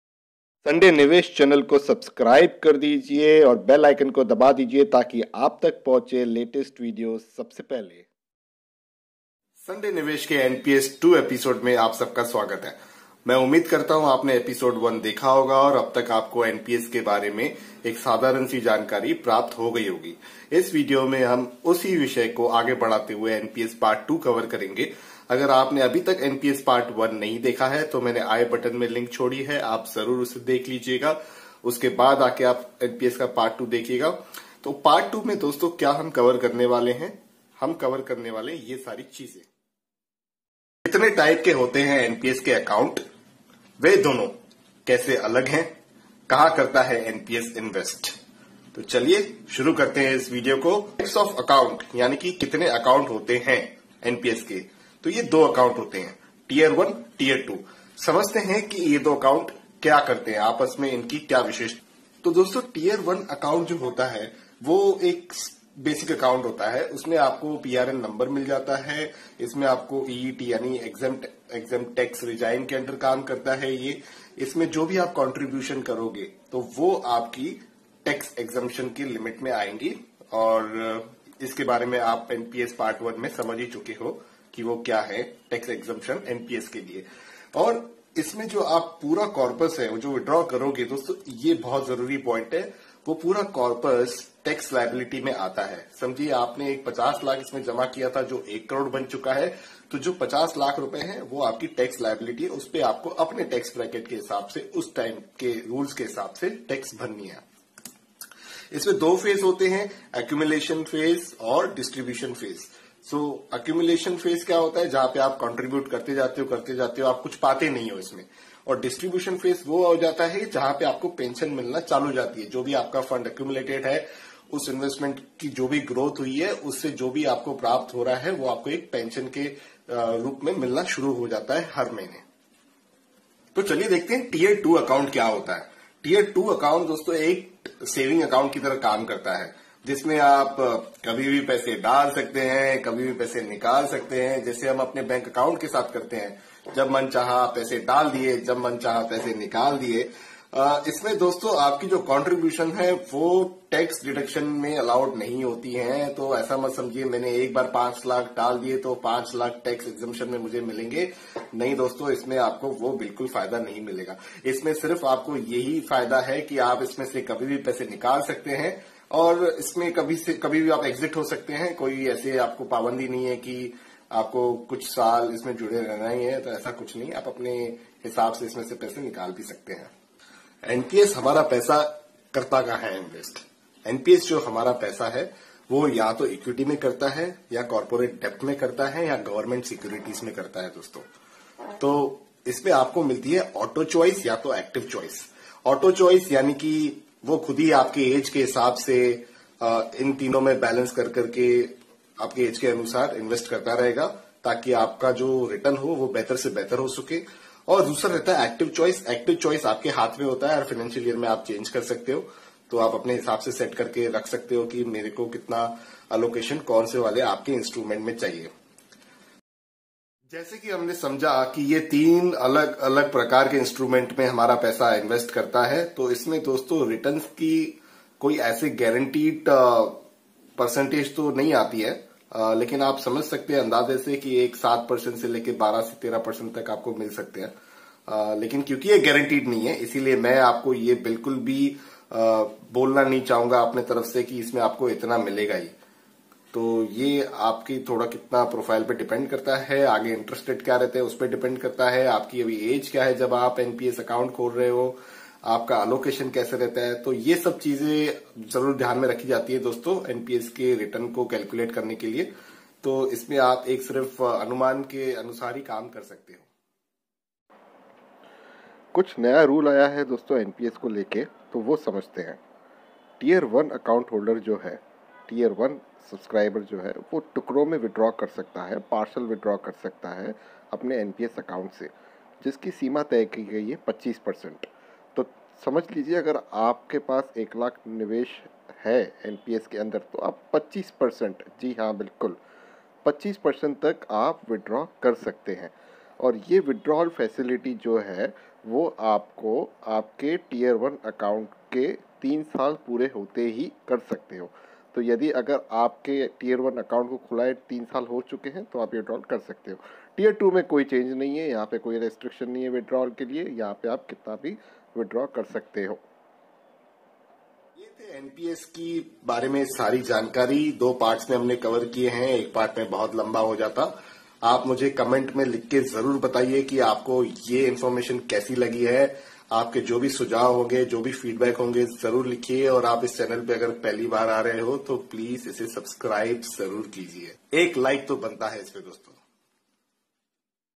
National pension System | NPS Tax Benefit | Pension scheme | Types of NPS accounts
National Pension System (NPS)?
The scheme allows subscribers to contribute regularly in a pension account during their working life. On retirement, subscribers can withdraw a part of the corpus in a lumpsum and use the remaining corpus to buy an annuity to secure

In Part 2, we will know about:
• Types of Accounts & Differences
• Where they invest
• Interest Rates
• Withdrawals with a new rule